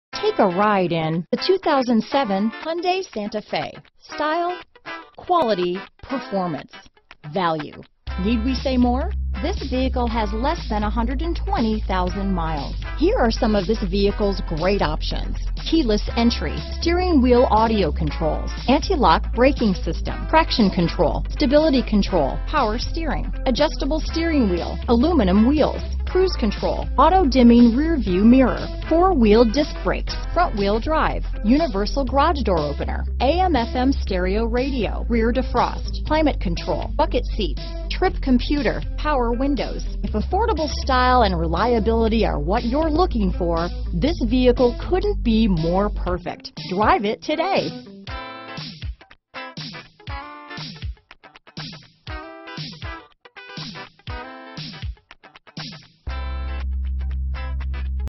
2007 Hyundai Santa Fe Smyrna, Marrietta, Atlanta, Alpharetta, Kennesaw, GA 333293A
Arctic White Used 2007 Hyundai Santa Fe available in Smyrna, Georgia at Ed Voyles Hyundai Kia. Servicing the Marrietta, Atlanta, Alpharetta, Kennesaw, GA area.
2007 Hyundai Santa Fe Limited - Stock#: 333293A - VIN#: 5NMSH13E67H016986